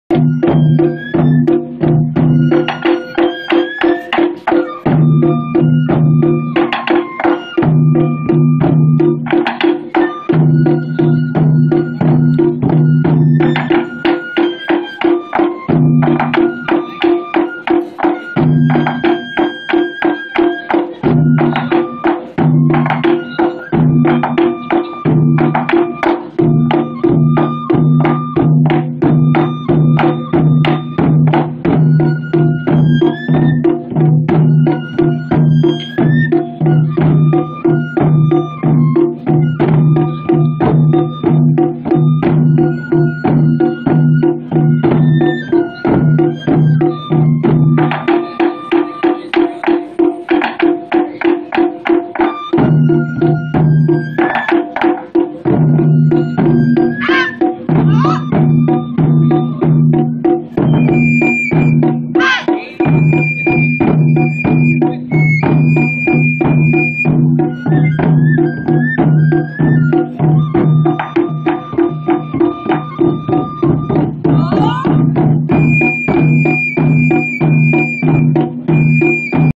Drumming at Steampunk
Traditional Japanese drumming at the Steampunk festival in Waltham